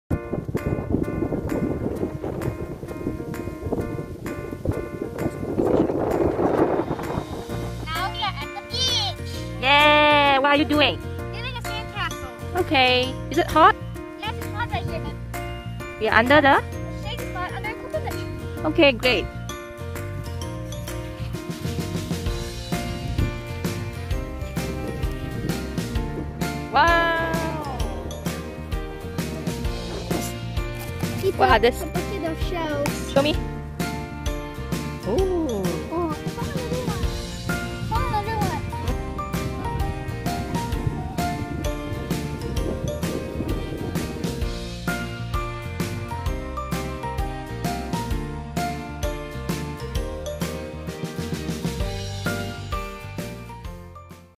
Sunny Day at Lexis hibiscus Beach
We had so much fun at Lexis Hibiscus beach: Building sandcastles, searching for pretty shells, playing with bubbles and flying kite!